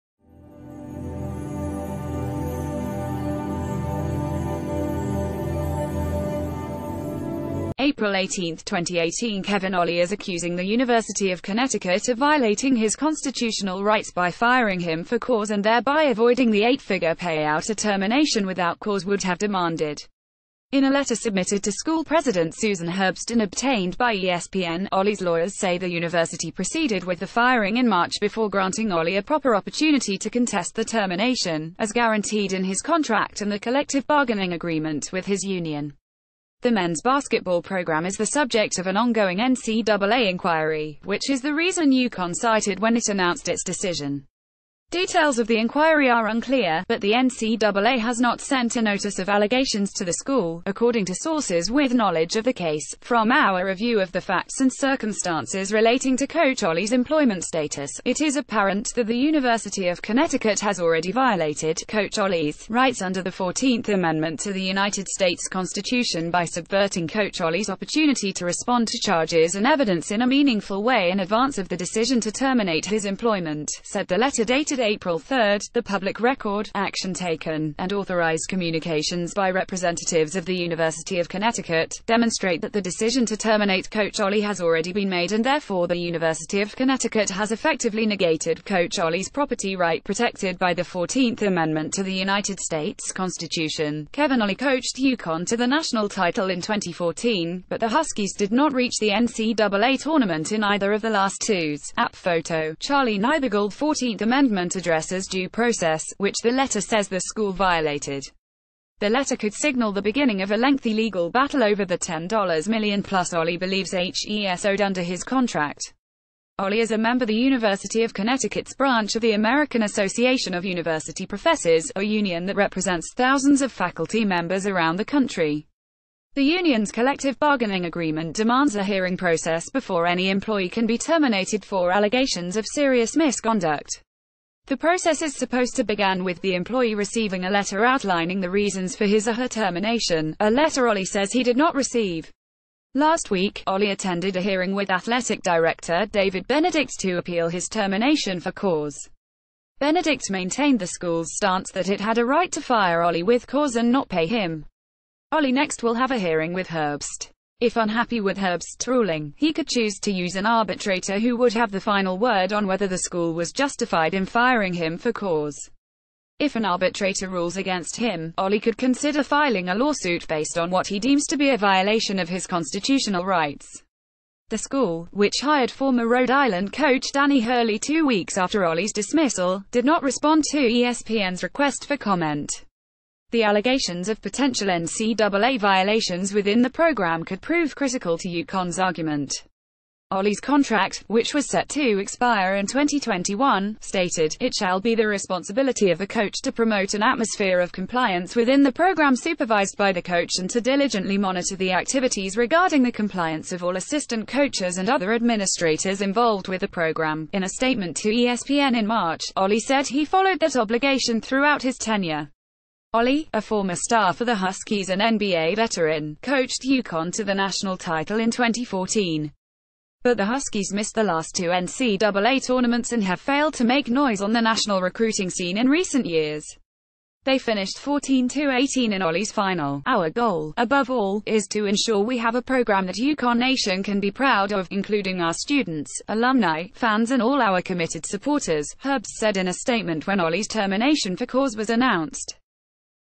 Kevin Ollie says University of Connecticut violated his rights by not giving him chance to contest
Kevin Ollie says University of Connecticut violated his rights by not giving him chance to contest firing